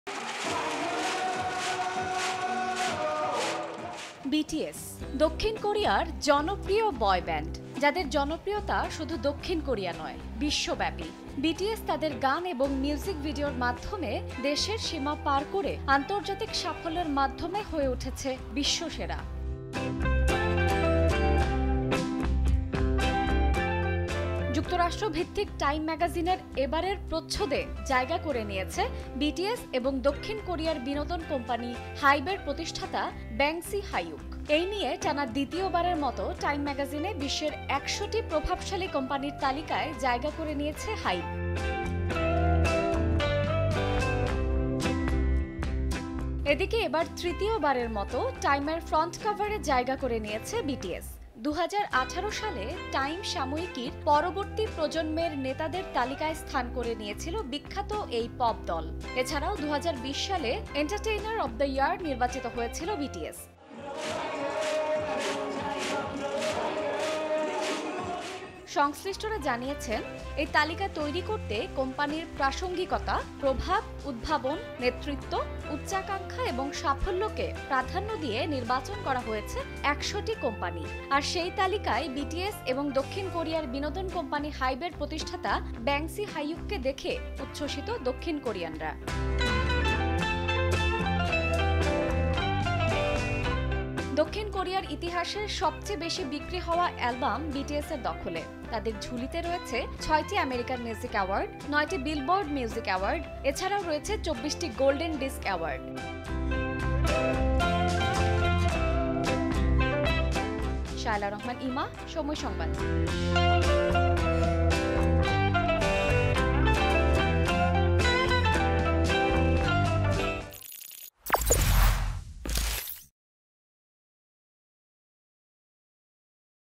টাইম ম্যাগাজিনের প্রচ্ছদে বিটিএস ও হাইব | BTS | Time | Hybe Corporation | Somoy Entertainment
'বিটিএস', দক্ষিণ কোরিয়ার জনপ্রিয় বয়-ব্যান্ড। যাদের জনপ্রিয়তা শুধু দক্ষিণ কোরিয়ায় নয়, তাদের পরিচিতি বিশ্বব্যাপী। বিটিএস তাদের গান এবং মিউজিক ভিডিওর মাধ্যমে দেশের সীমা পার করে আন্তর্জাতিক সাফল্যের মাধ্যমে হয়ে উঠেছে বিশ্বসেরা।